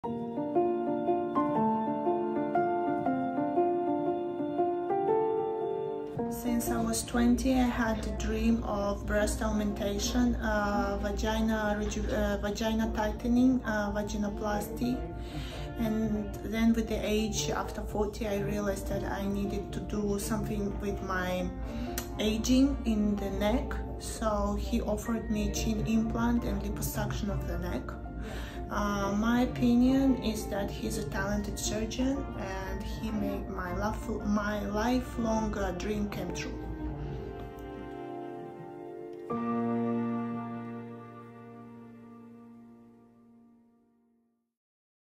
Thankful For Our Amazing Patients That Share Their Experience At Vitenas Cosmetic Surgery!
We are so grateful to have patients that want to share their journey!

Dr. Vitenas offers complimentary consultations in Houston, TX.

Vitenas Cosmetic Surgery
4208 Richmond Ave. Suite 200